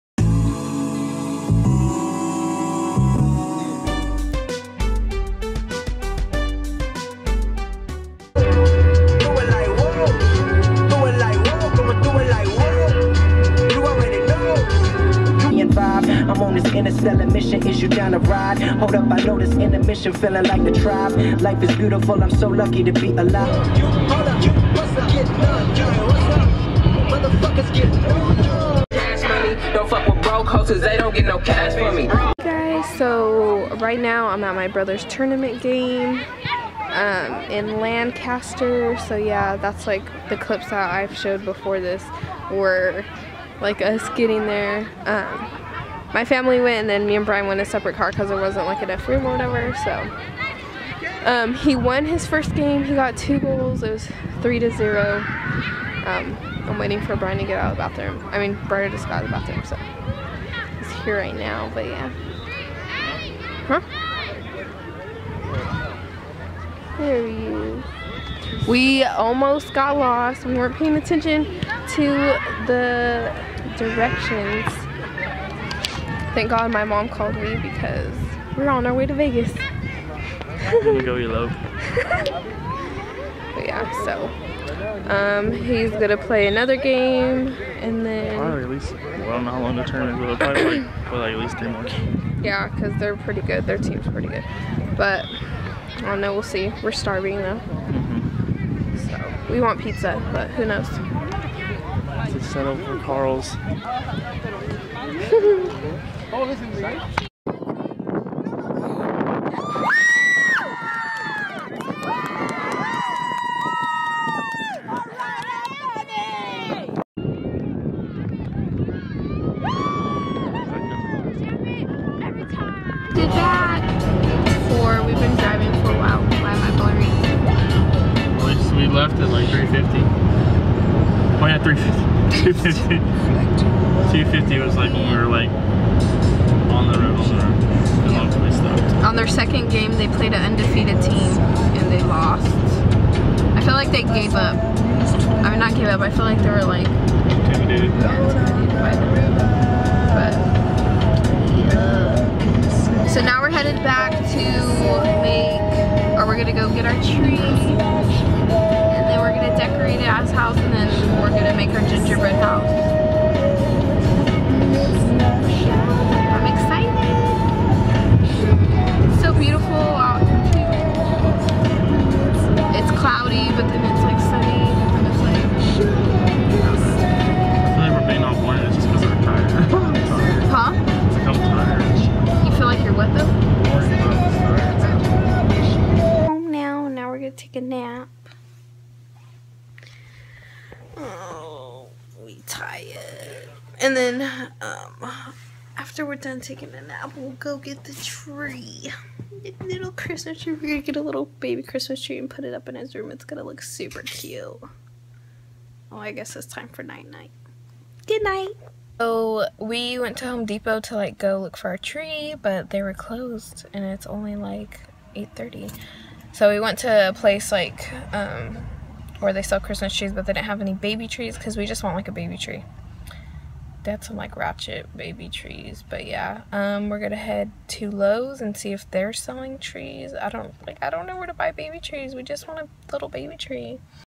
Vlogmas Day 12: MeggMAS 2015
Today was my brother soccer tournament. Come along with Bryan and I as we travel to Lancaster. Did we find our baby Christmas Tree?